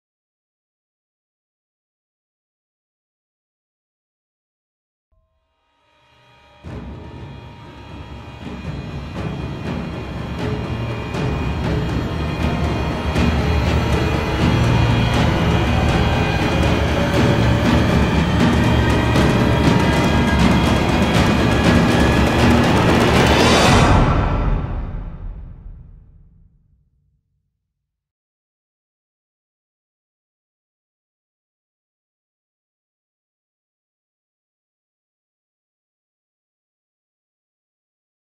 SWIFF16 Film Trailer
SWIFF is an International Film Festival featuring live guests and Artist workshops. SWIFF will open its doors in Sarnia, Ontario this November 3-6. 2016.